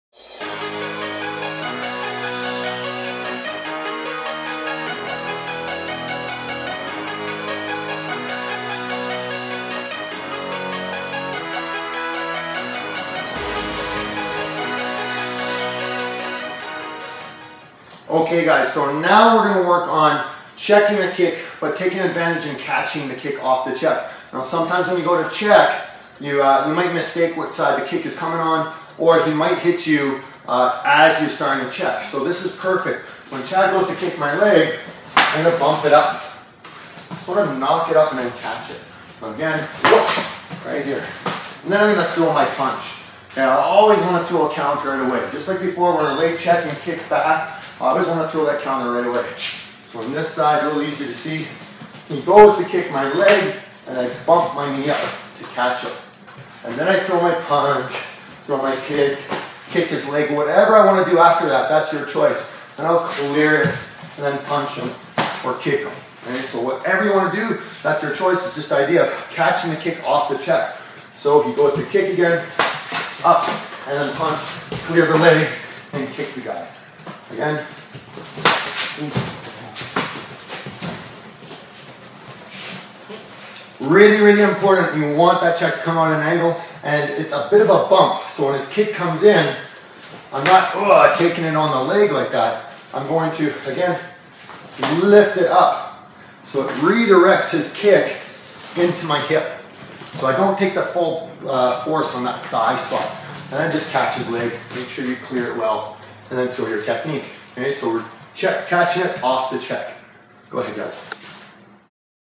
Low 3 - Check Catch
Super awesome move and one of my favorites.  If you like catching Body Kicks, you'll absolutely love this one.